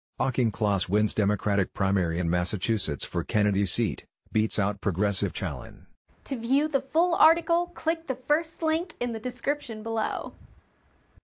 Auchincloss wins Democratic primary in Massachusetts for Kennedy seat, beats out progressive challen
Jake Auchincloss, a USMC veteran with a Republican history, eked out a victory in the Democratic primary for a Massachusetts House seat after an heavily-contested three way race that started with nine different candidates vying to replace the congressional seat of Joe Kennedy III, according to a projection Friday by the Associated Press. The election was held Tuesday and the results showed Auchincloss, 32, winning with just 22. 3% of the vote and a margin of a little over 2,000. In the end too many candidates each with major backing by a different faction of the progressive caucus canceled each other out and allowed Auchincloss, who once worked as an operative for former Republican operative for Gov. Charlie Baker. Im honored that the people of MA04 have chosen me as the Democratic nominee for Congress. We won 25 of the 34 cities and towns across the district, a testament to the strong, full-district campaign we built, tweeted Auchincloss, who dominated the more rural and working class townships that border Rhode Island to find the narrow path to victory. KENNEDY CONCEDES TO MARKEY IN HEATED MASSACHUSETTS SENATE DEMOCRATIC PRIMARYJesse Mermell, the former communications director to Gov. Deval Patrick and local city councilor who was endorsed by Squad member Rep. Ayanna Pressley followed with 21% of the vote. Becky Grossman, a member of Newton city council endorsed by Rep. Ro Khanna, a senior adviser to Sanders 2020 campaign, received 18% of the vote. Natalia Linos, an epidemiologist who ran a campaign focused on building healthy communities and advocating for social justice through reparation received 11% of the total vote. Ihsane Leckey, a former regulator at The Federal Reserve endorsed by Squad member Rep. Ilhan Omar came in 5th with 11% of the vote. The combined vote totals of the other progressive candidates would have easily defeated Auchincloss in a district which has most recently been represented by liberal stalwarts